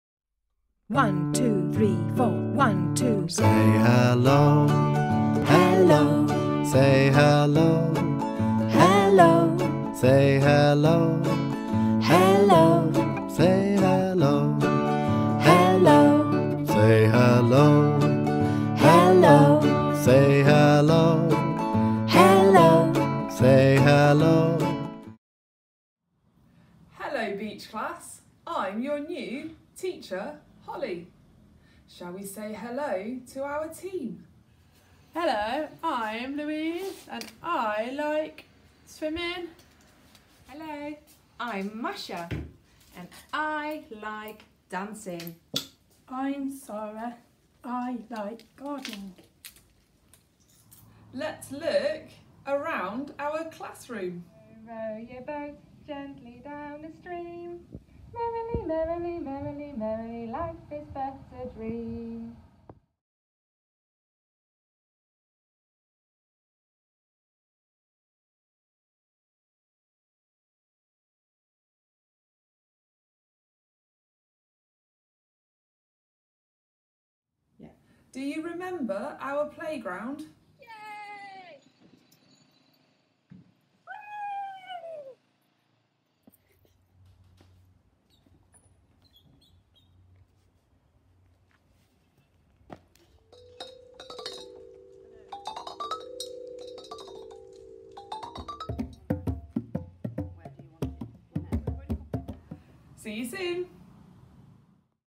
Beech Class 2021 2022
Come and have a look around Beech Class for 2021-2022. I look forward to seeing you in September!